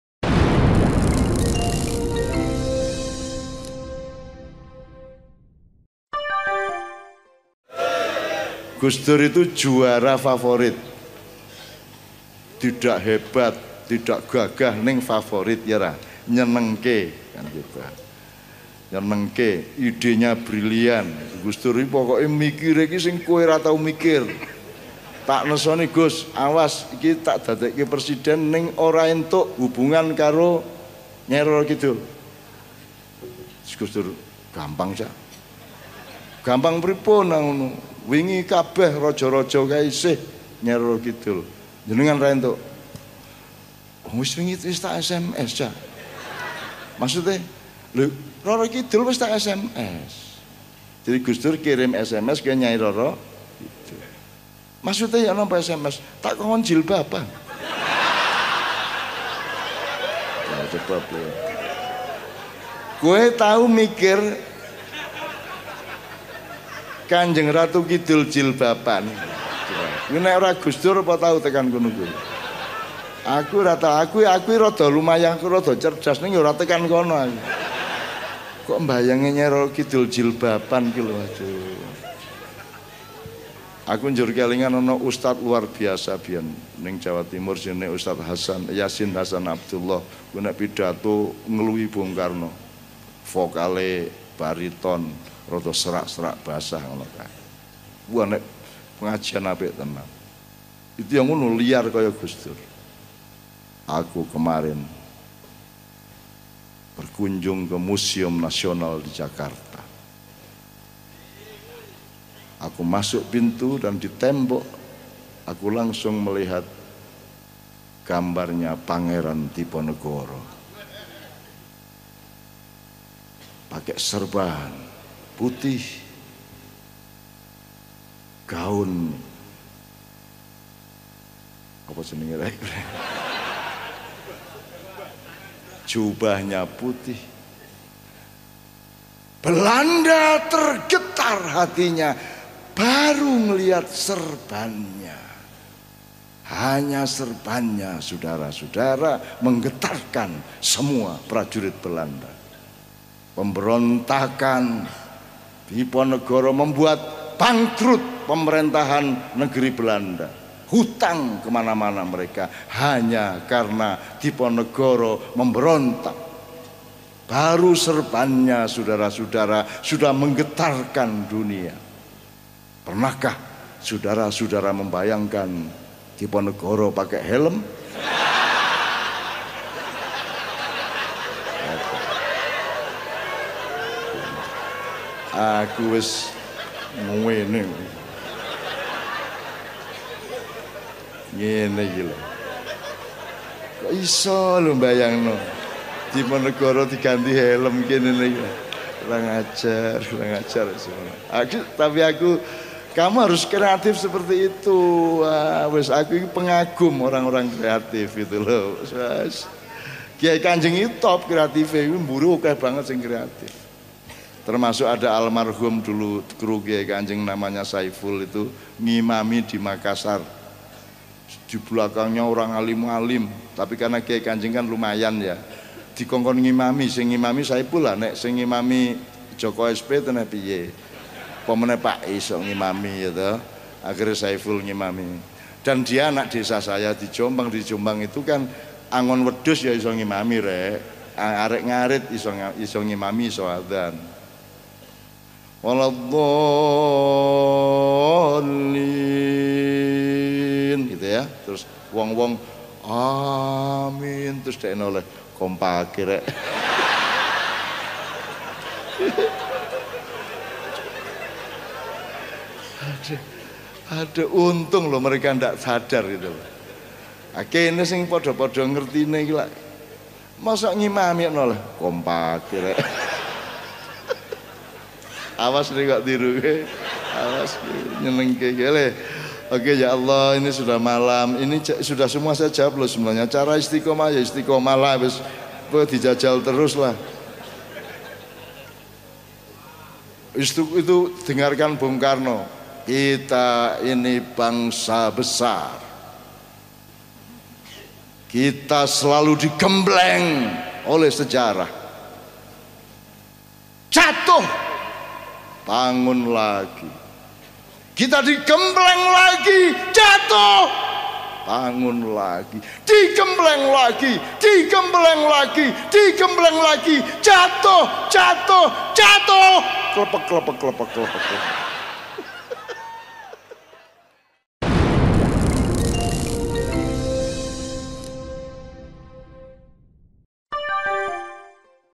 Gus Dur Kirim Surat Nyai Roro Kidul, Isinya Membuat Cak Nun Takluk Tak Berkutik | Bangkit TV
Gus Dur Kirim Surat Nyai Roro Kidul, Isinya Membuat Cak Nun Takluk Tak Berkutik

Kisa Cak Nun ini mendebarkan, sekaligus membuat tawa kita semua. Ketika jadi Presiden, Gus Dur diminta Cak Nun agar tidak bercengkrama dengan Nyai Roro Kidul. Tapi malah dijawab Gus Dur kalau sudah kirim surat (SMS) kepada Nyai Roro Kidul. Seketika Cak Nun takluk, tak berkutik.

Apa isi surat Gus Dur? Ternyata Gus Dur malah kirim surat agar Nyai Roro Kidul pakai kerudung. hehehe. Cak Nun makin takjub dengan Gus Dur. Apa yang dipikirkan Gus Dur memang melampaui alam pikiran manusia Indonesia pada umumnya.

Banyak hikmah dari kisah ini. Selain membuat tawa, sekaligus menjadi media kritik atas diri manusia Indonesia.

Selengkapnya Silahkan simak didalam Vidio ini.
Salam Bangkit TV